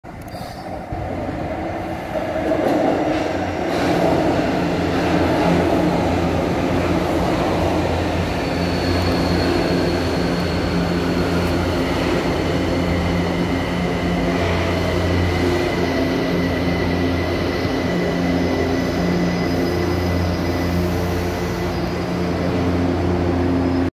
Frota J (J38) chegando na estação Tamanduateí
TUE J38 (Ex A38 - Bombardier) chegando sentido Vila Prudente